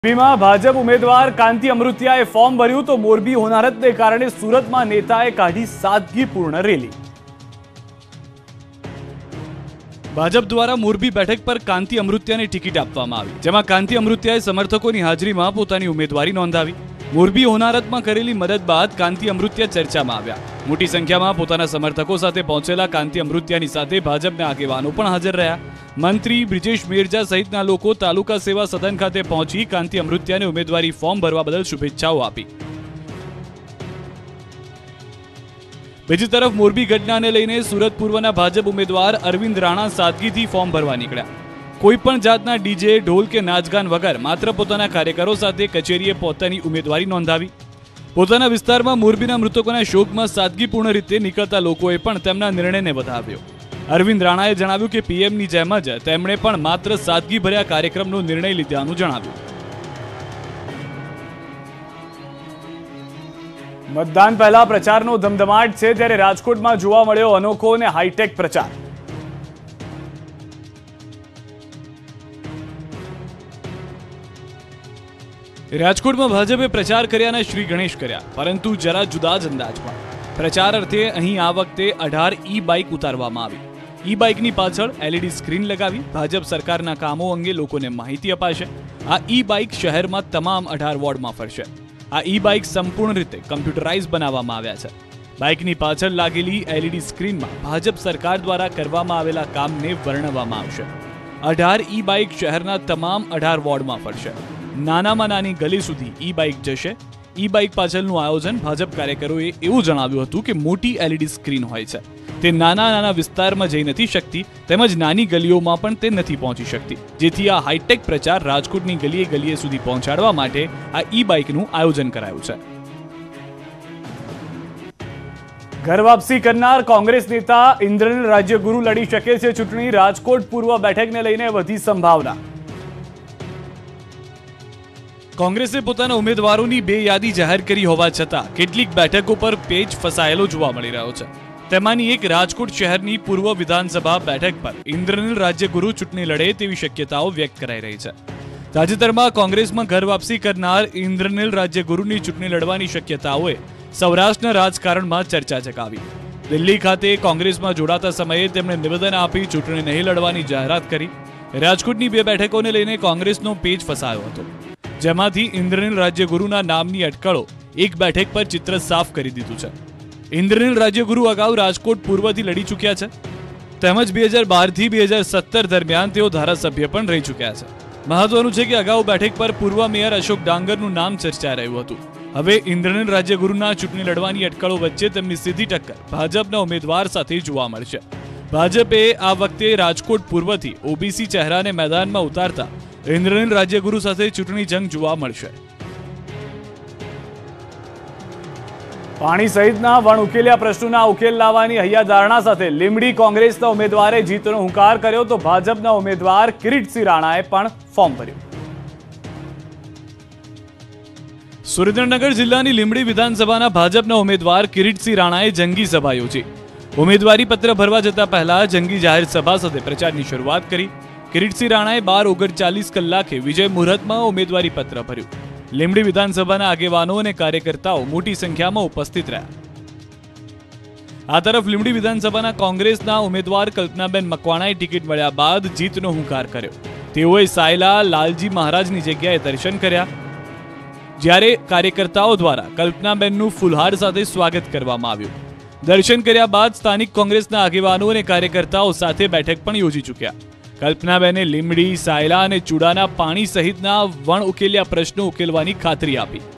Surat News । નેતાએ સાદગીથી કાઢી રેલી | BJP candidates | Gujarat news
Enthusiasm was seen among the BJP candidates

ભાજપના ઉમેદવારોમાં ઉત્સાહ જોવા મળ્યોn18oc_Surat